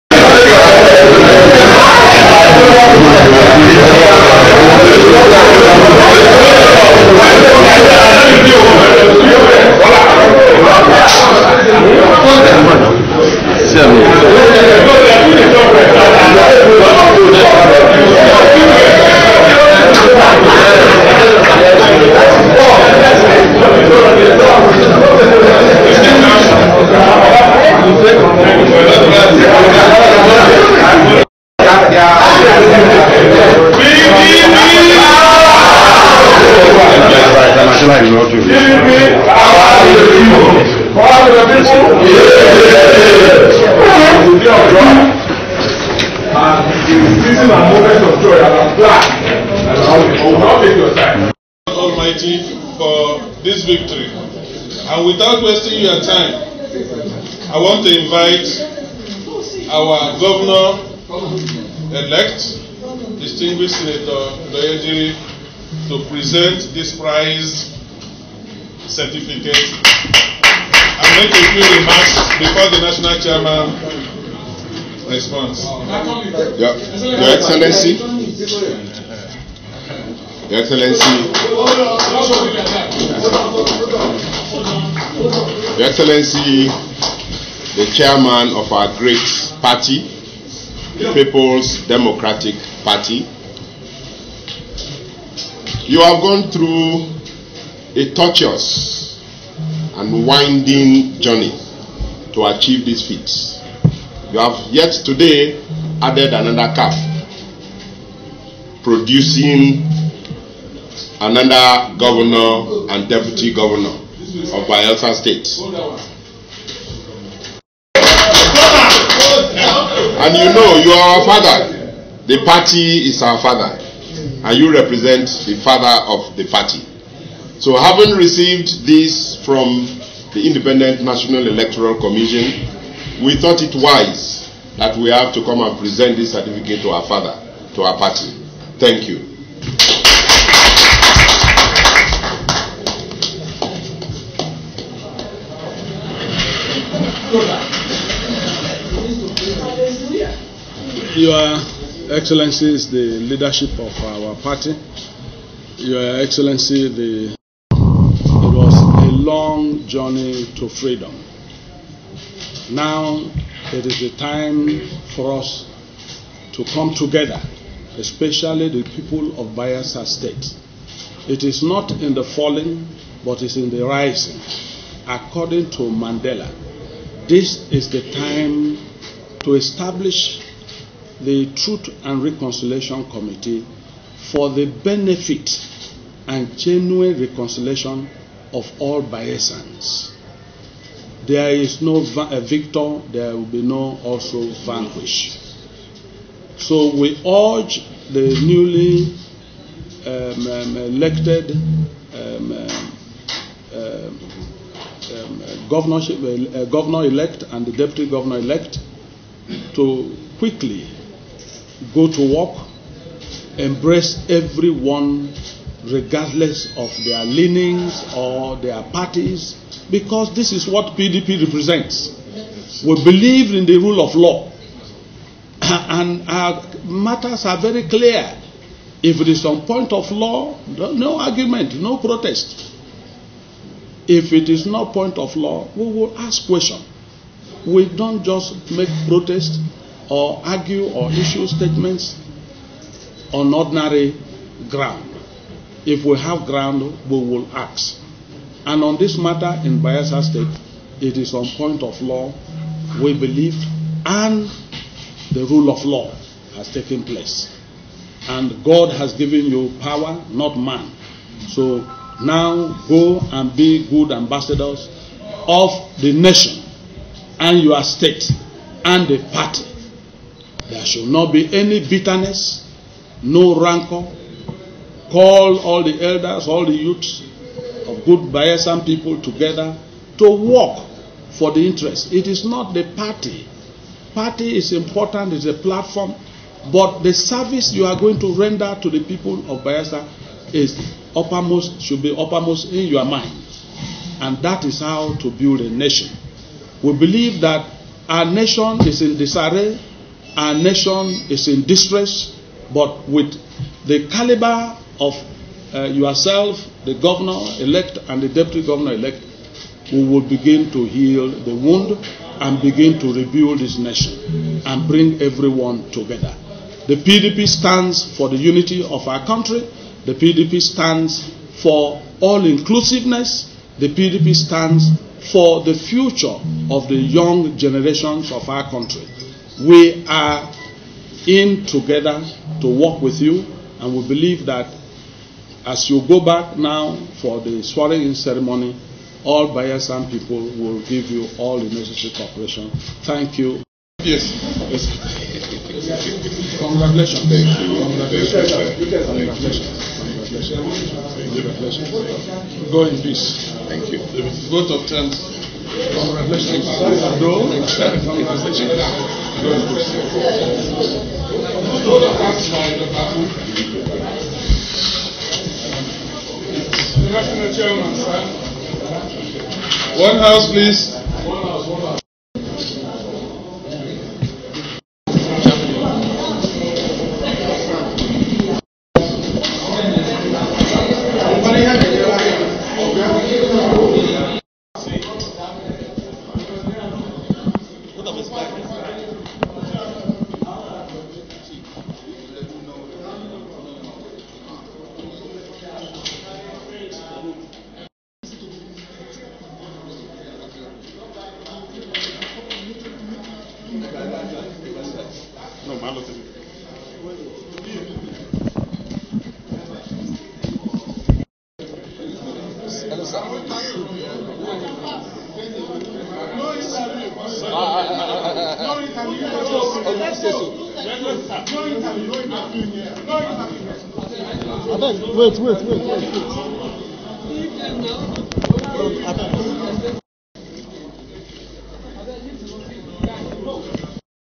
FULL VIDEO : Senator Douye Diri Present His Certificate of Return to PDP National Leadership
PDP Candidate and his able Deputy are at the INEC now for their Certificate of Return as Governor of Bayelsa State